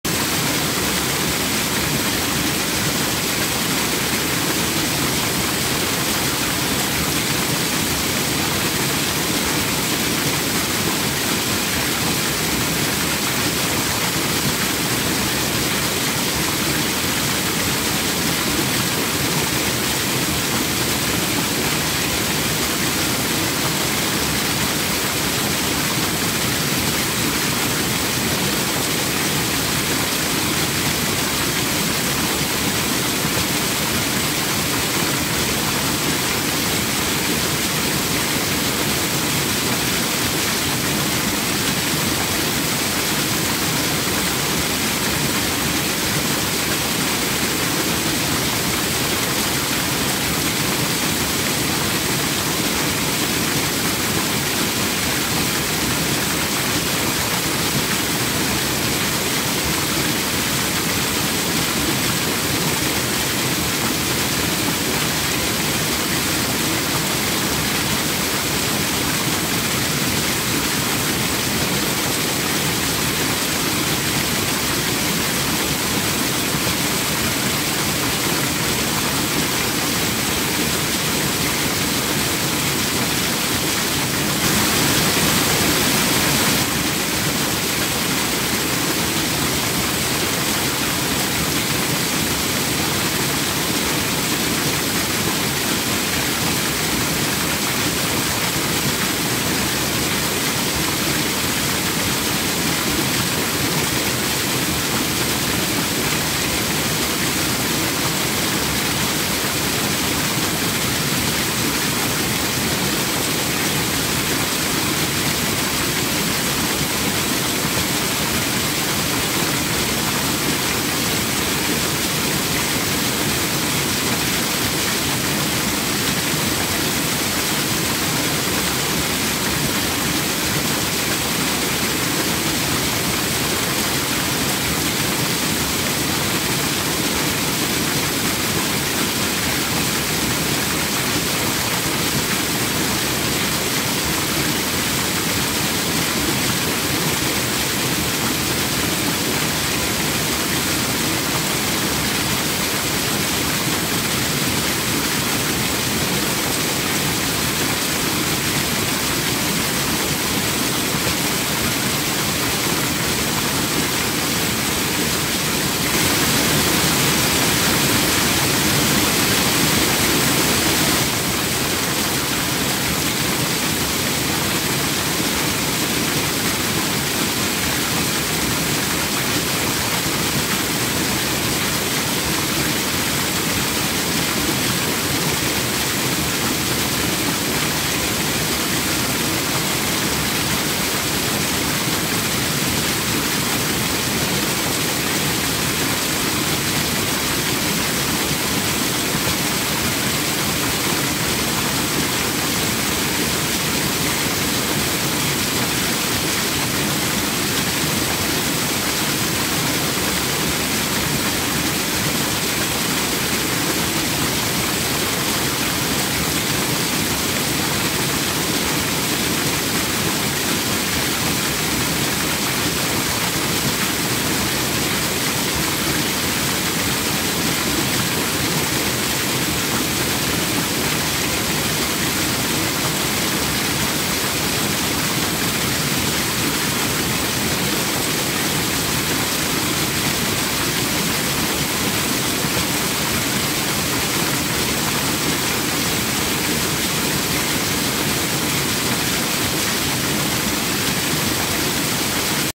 Relaxing Music with Nature Sounds - Waterfall HD | Loop for Relaxing-Relaxing Maditation Music
Relaxing Music with Nature Sounds - Waterfall HD | Loop for Relaxing-Relaxing Maditation Music and nature

Hello everyone on YouTube! Today, I came here for you becuase Today, every person is in some kind of trouble, so I have made a video to end this trouble, after listening to it, you will get rid of your worries and you will feel calm.